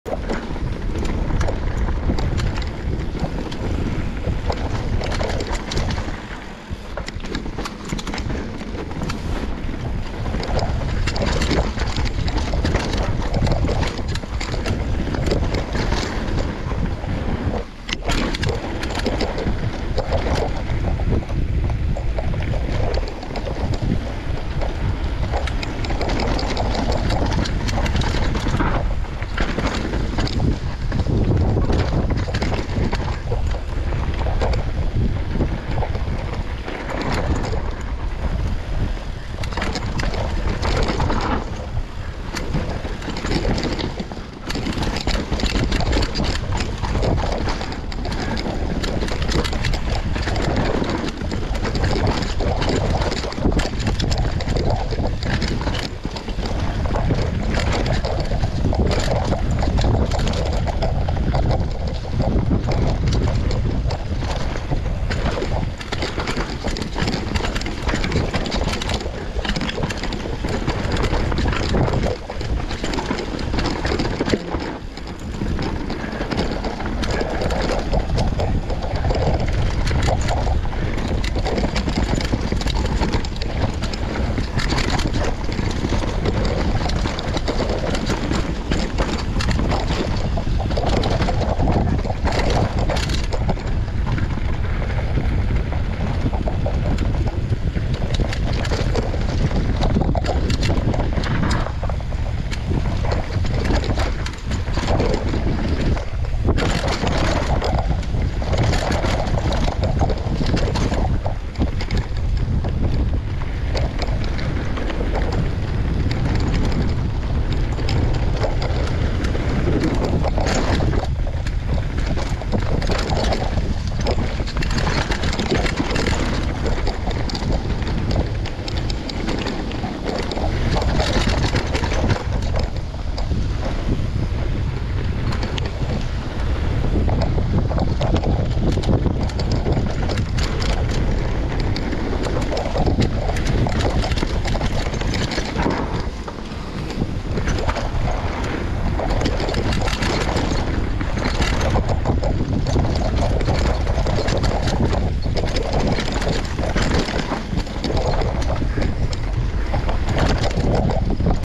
Havoc - Sugarloaf Mountain Bike Park, New Brunswick
Havoc is a Black Diamond steep rooty, rocky and technical descent. Ride with caution.